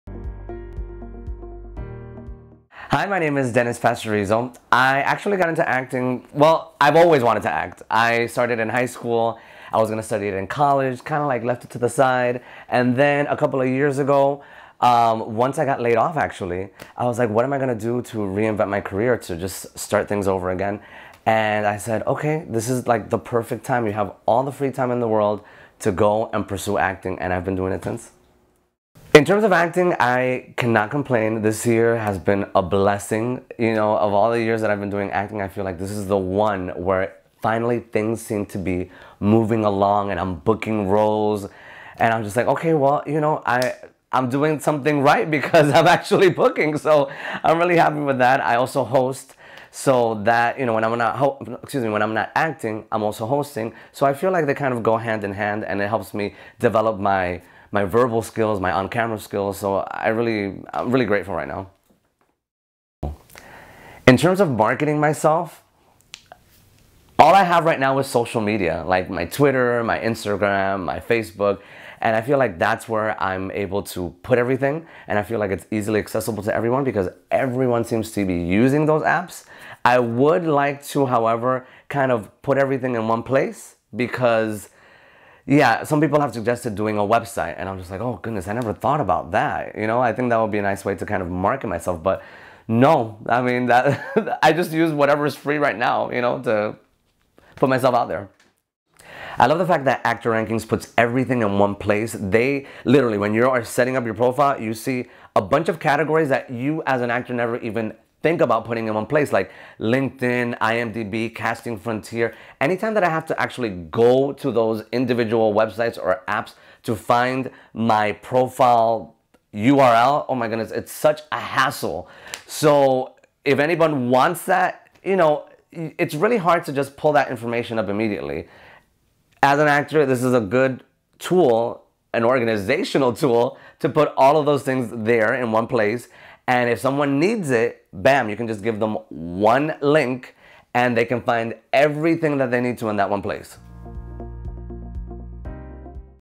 I want to become an actor where do I start - Actor Rankings
Meet Dennis Pastorizo a professional actor living in Los Angeles and pursuing his career. Dennis tells us how he got started in the industry as a professional actor.  Actor Rankings is dedicated to helping actors with their social media rankings and giving them a voice and a face for potential casting opportunities.